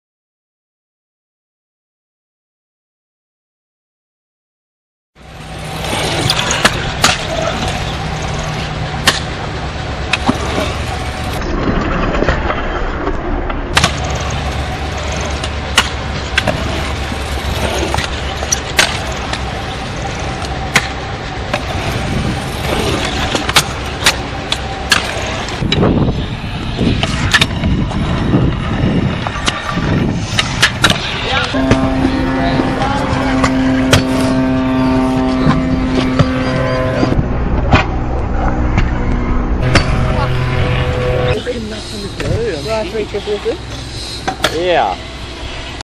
Corel Video Studio Test
I just downloaded Corel Video Studio Pro x3 and i'm trying to use it lol. Most of these clips are ones which were supposed to be in vids but weren't, so pretty much throwaway. Comment if you like this more than my older vids or if yo have any ideas for something that i should change.